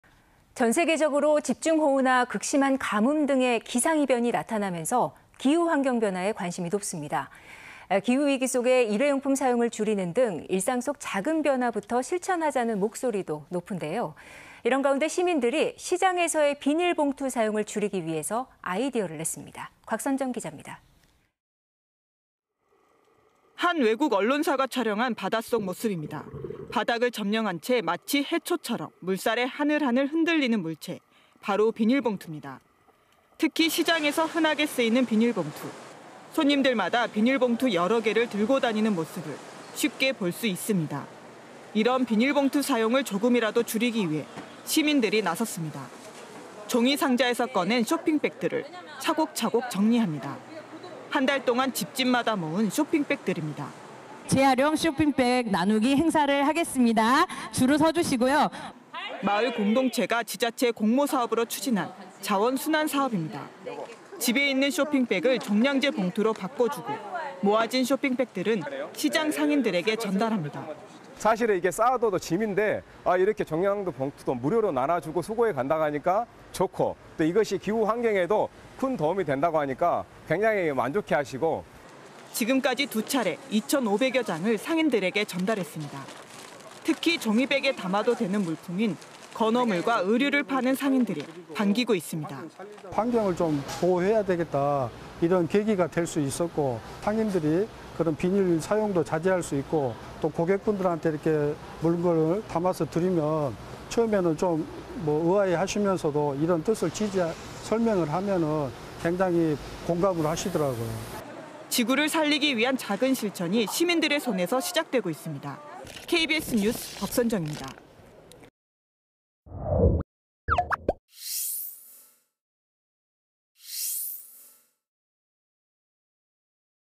“비닐 봉투 대신 재활용 쇼핑백”…기후위기 작은 변화부터 / KBS 2023.07.21.
[앵커]

 최근 집중호우 같은 기상이변은 기후변화와도 관계가 있죠.

 플라스틱이나 비닐 같은 일회용품 사용은 기후변화를 부채질하고 있는데요.

 시민들이 시장에서 비닐봉투 사용을 조금이라도 줄이기 위해 아이디어를 냈습니다.

 곽선정 기자입니다.

 [리포트]

 한 외국 언론사가 촬영한 바닷속 모습입니다.

 바닥을 점령한 채 마치 해초처럼 물살에 하늘하늘 흔들리는 물체, 바로 비닐봉투입니다.

 특히 시장에서 흔하게 쓰이는 비닐봉투.

 손님들마다 비닐봉투 여러 개를 들고 다니는 모습을 쉽게 볼 수 있습니다.

 이런 비닐봉투 사용을  조금이라도 줄이기 위해 시민들이 나섰습니다.

 종이상자에서 꺼낸  종이 쇼핑백들을 차곡차곡 정리합니다.

 한 달 동안 집집마다 모은 쇼핑백들입니다.

["재활용 쇼핑백 나누기 행사를 하겠습니다. 줄을 서주시고요."]

 마을공동체가  지자체 공모사업으로 추진한  자원순환 사업입니다.

 집에 있는 쇼핑백을  종량제봉투로 바꿔주고 모아진 쇼핑백들은 시장 상인들에게 전달합니다.

 [강승철/화기애애 화정마을기후 수호단 : "사실 쌓아두어도 짐인데 종량제봉투도 무료로 나눠주고 수거해간다 하니까 좋고 이것이 기후 환경에도 큰 도움된다하니까 굉장히 만족해하시고."]

 지금까지 두 차례, 2천5백여 장을 상인들에게 전달했습니다.

 특히 종이백에 담아도 되는 물품인 건어물과 의류를 판매하는 상인들이 반기고 있습니다.

 [이명근/양동건어물시장 상인회장 : "환경을 보호해야겠다 이런 계기가 될 수 있었고 상인들이 비닐 봉투를 자제할 수 있고 또 고객분들한테 물건을 담아서 드리면 처음엔 의아해하시면서도 이런 뜻을 설명하면 굉장히 공감하시더라고요."]

 지구를 살리기 위한 작은 실천이 시민들의 손에서 시작되고 있습니다.

 KBS 뉴스 곽선정입니다.

 촬영기자:이성현